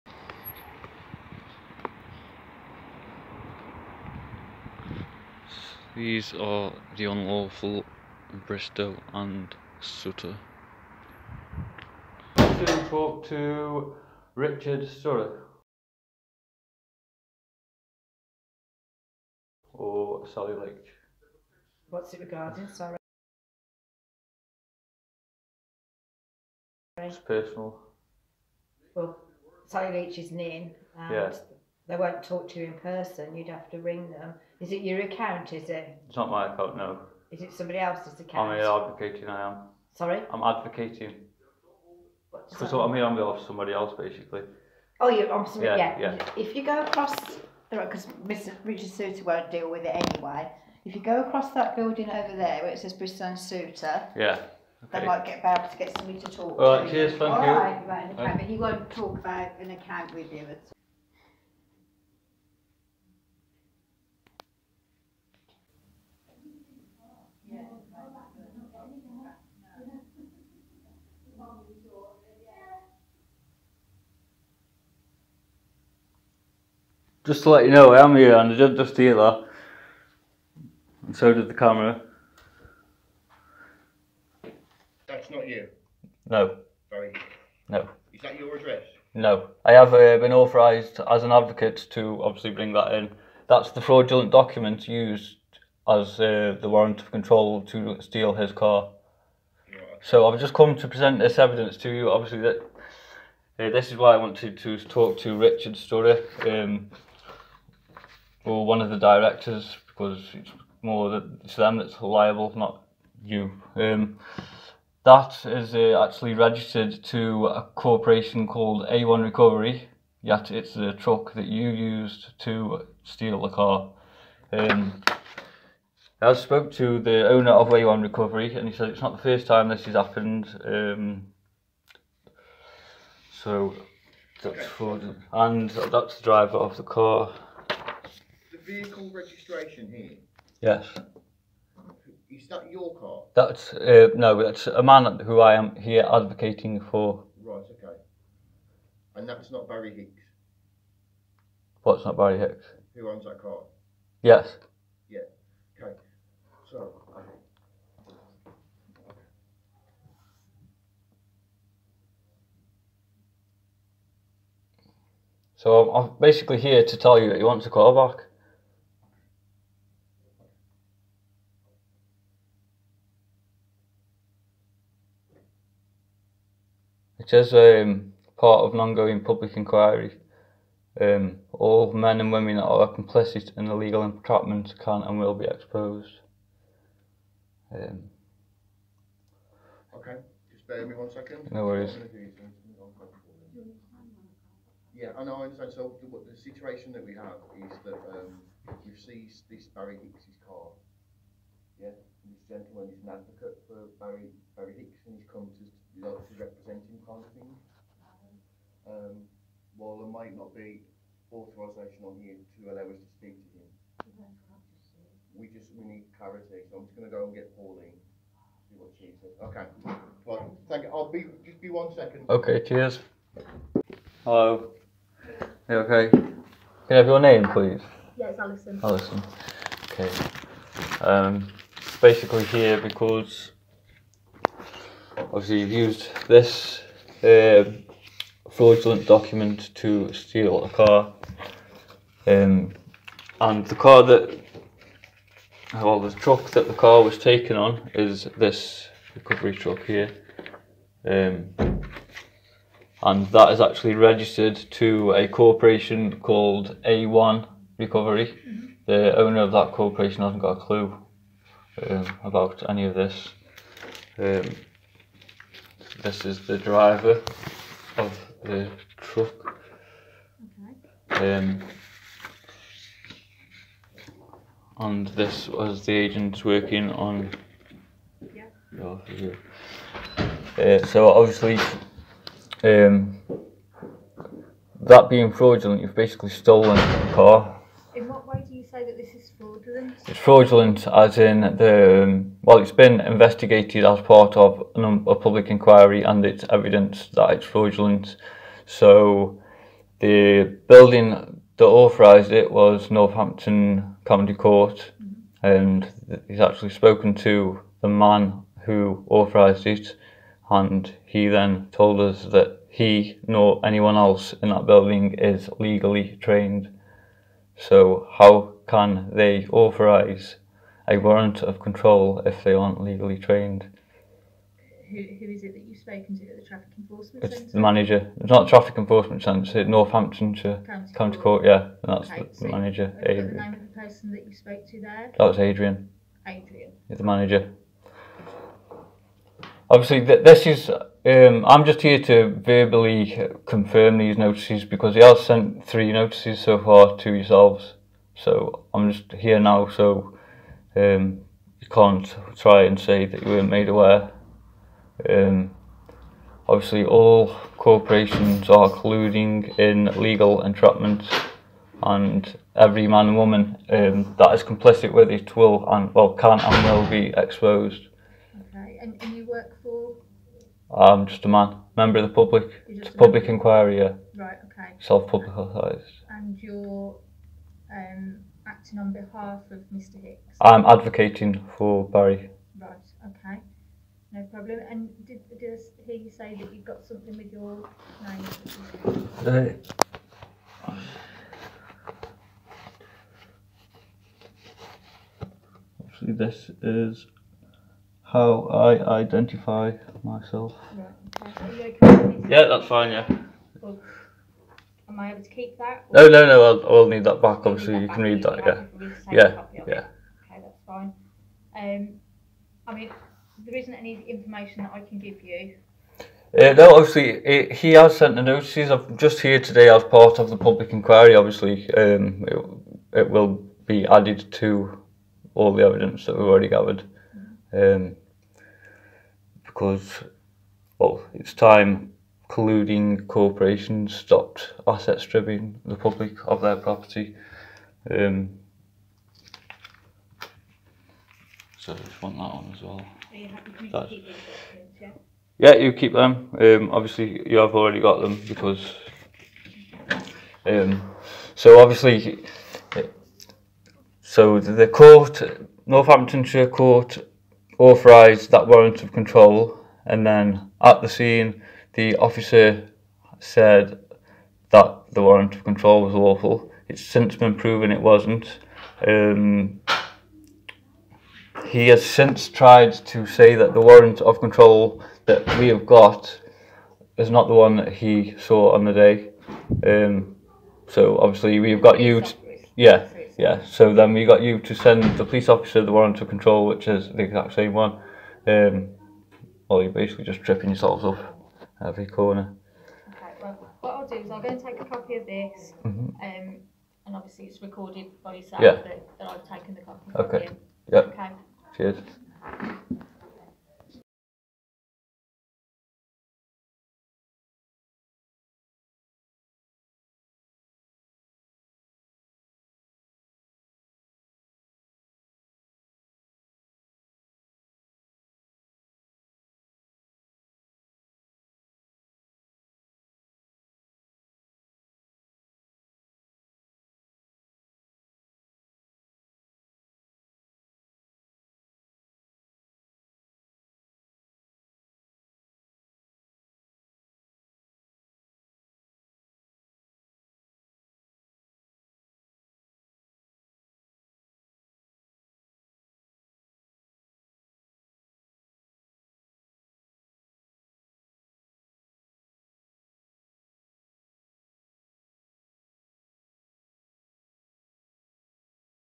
Exposing Bristow and Sutor- public enquiry video 3.
Alison Marshall supervisor with 30 years  experience said with a colleague
 "The  long  and  short of  it"  they're  not  gonna  get the  "car  back.         ha ha ha"

Bristow and Sutor have now been giving evidence verbally and written with notices.

like to say a big thank you for Sam for Calling these people out and giving your time but for now I know there are  alot of people very frustrated but PLEASE hang off for now.

please make sure you watch video 1 and video 2